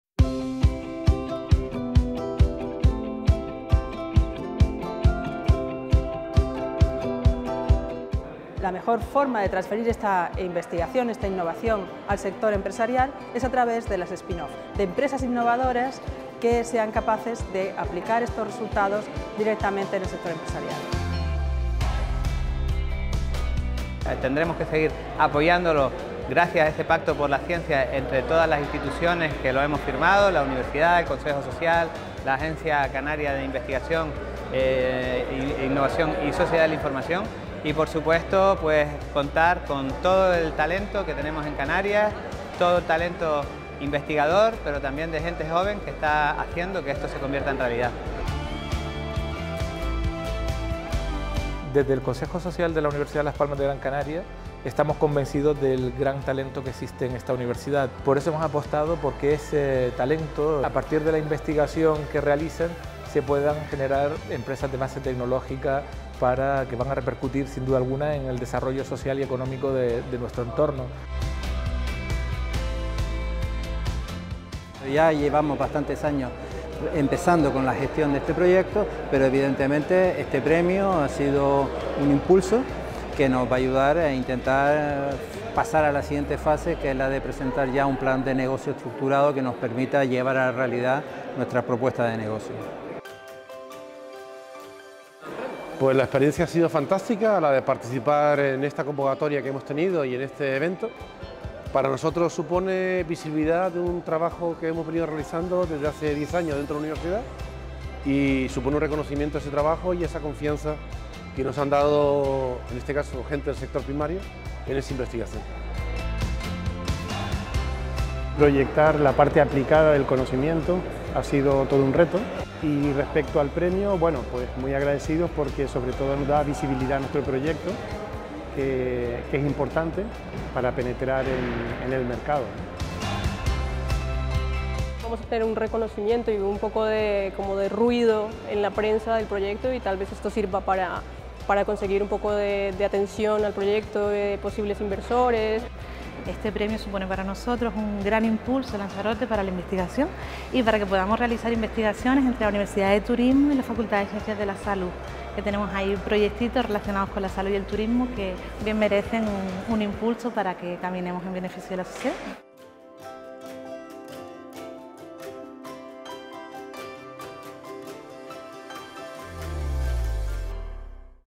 Demo Day, SPIN ON by ULPGC, 21 de diciembre de 2022
El día 21 de diciembre de 2022 tuvo lugar la presentación de potenciales spin offs académicas gestadas en la primera fase de la iniciativa SPIN ON by ULPGC.

Se concedieron hasta 5 “Premios de la idea al negocio” por un valor de 2.500€ cada uno

El evento tuvo lugar el miércoles 21 en horario de mañana en el espacio NEXO Espacio Innovación de la ULPGC, en el Campus de Tafira.